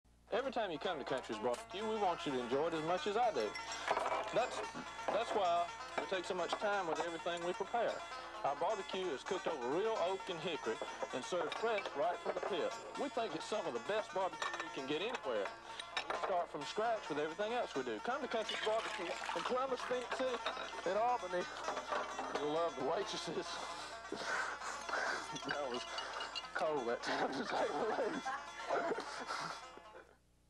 "The Waitress" Country's BBQ (ca. 1979)
Hilarious. Owner,Jim Morpeth in local Columbus, Georgia TV spot with hap-hazzard waitress. Now Open in Auburn. Produced by Jack Basset.